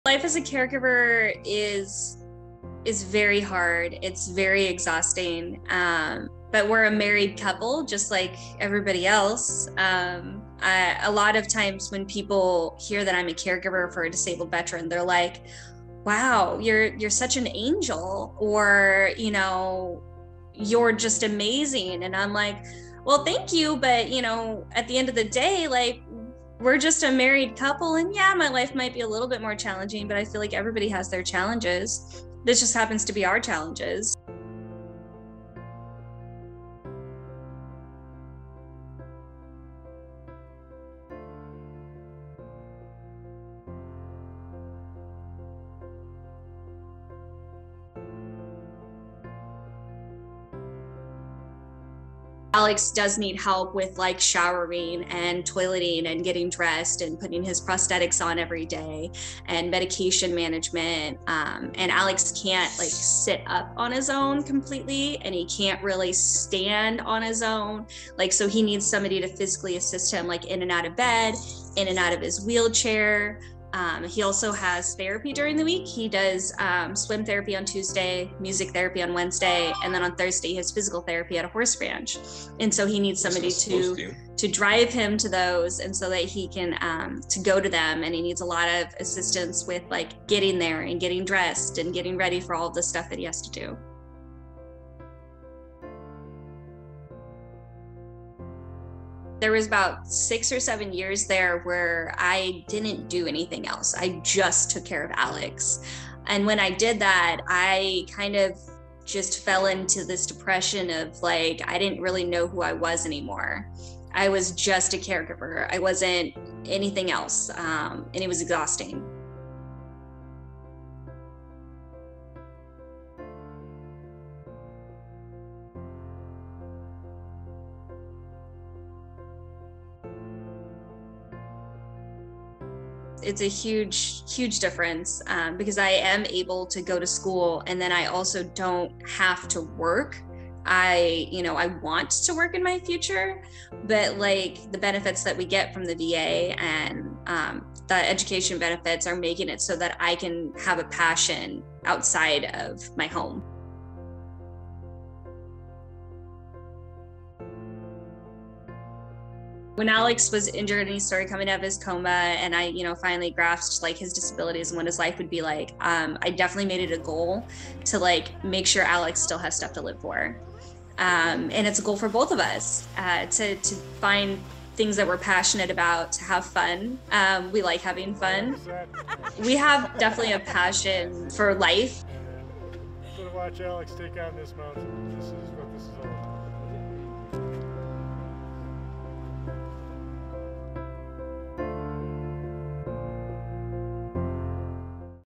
Alex and Kim Hussey's Caregiver Story
Alex lost both legs and most of his left hand after stepping on an IED in 2012. Kim went from college student to caregiver. Thanks to VA caregiver benefits, she was able to go back to school and pursue a career.